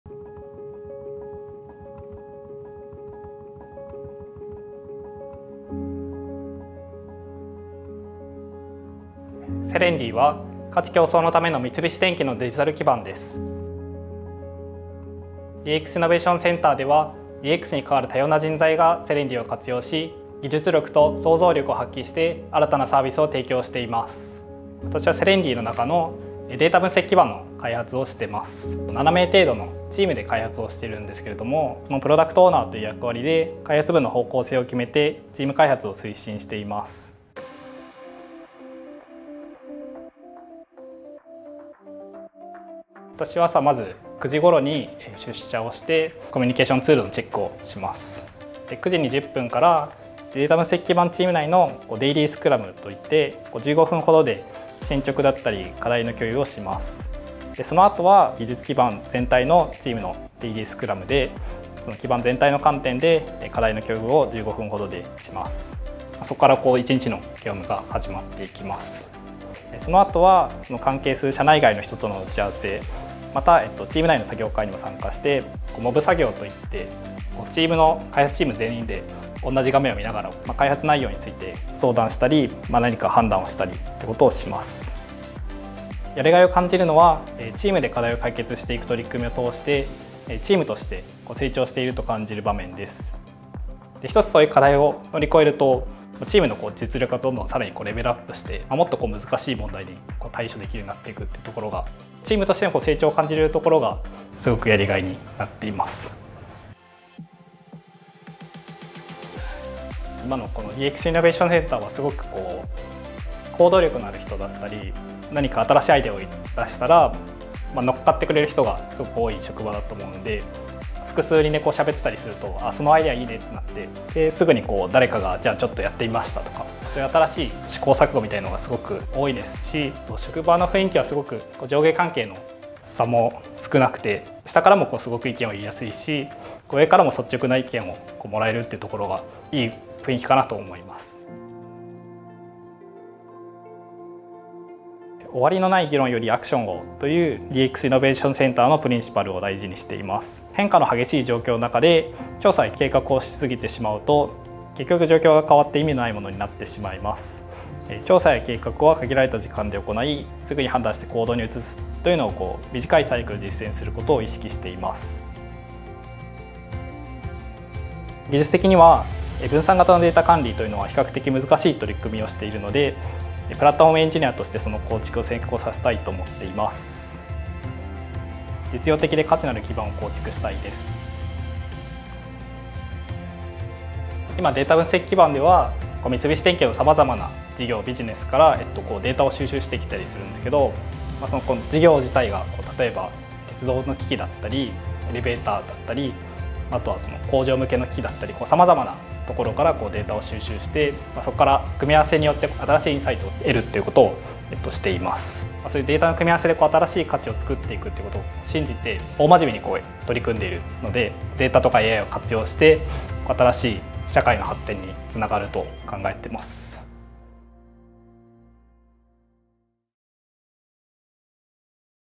三菱電機が進める価値創出の最前線で活躍する「DXエンジニア」の仕事とは
「終わりのない議論より、結果の出るアクションを」

三菱電機DXイノベーションセンター（DIC）が掲げる12のプリンシパルの中でも、この言葉を大事にする古跡進さん。判断と行動を短いサイクルで実践するよう心がけ、様々なデータと人の出会いによる価値創出を目指す。三菱電機グループのこれからを担う「DXエンジニア」として働く姿に迫った。